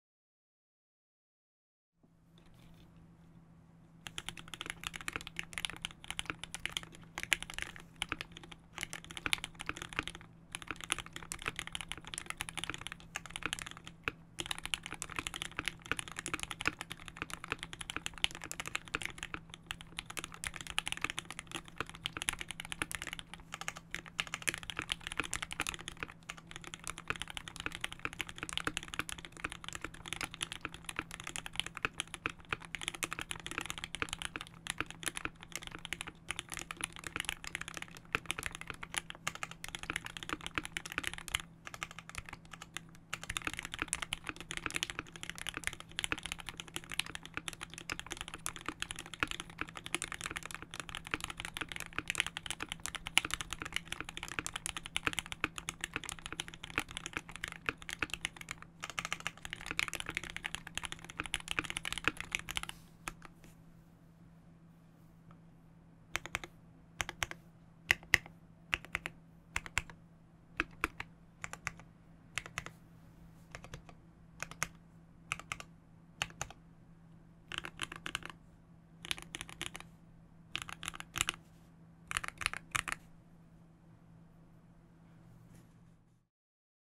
Lin Montage with Gateron Giants (Sandwich Mounted) Typing Test
Keyboard: Peacock Lin Montage w/ Shelf Liner
Plate: 2T Brass
Mounting: Sandwich
Keycaps: CRP Tulip
Switches: Gateron Giants
Lube: Krytox 205g0
Stabs: Durock